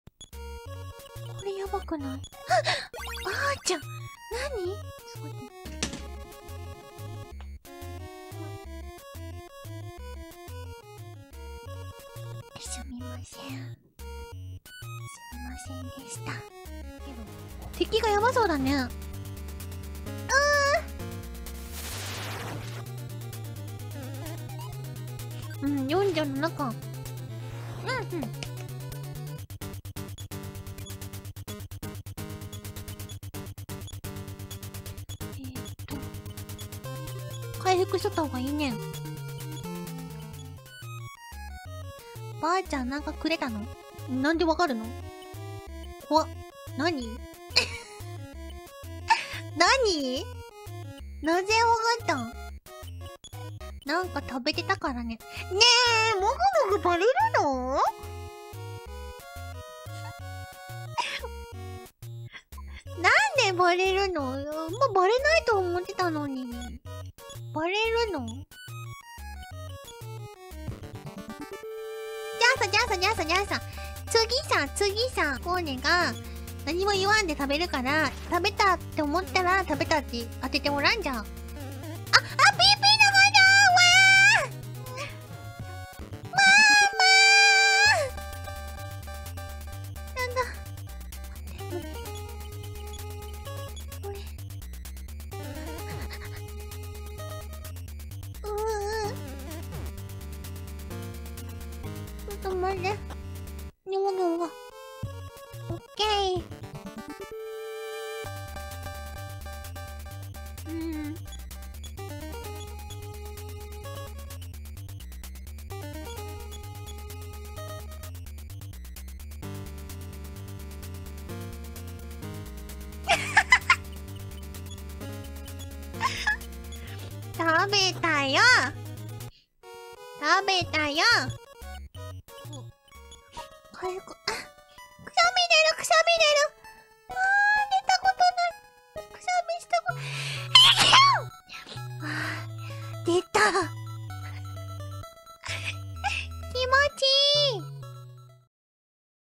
Korone Immediately Gets Caught Trying to Secretly Eat While Streaming [Hololive]
Grandma Inugami brings Korone some snacks while she's streaming, and immediately gets called out by chat.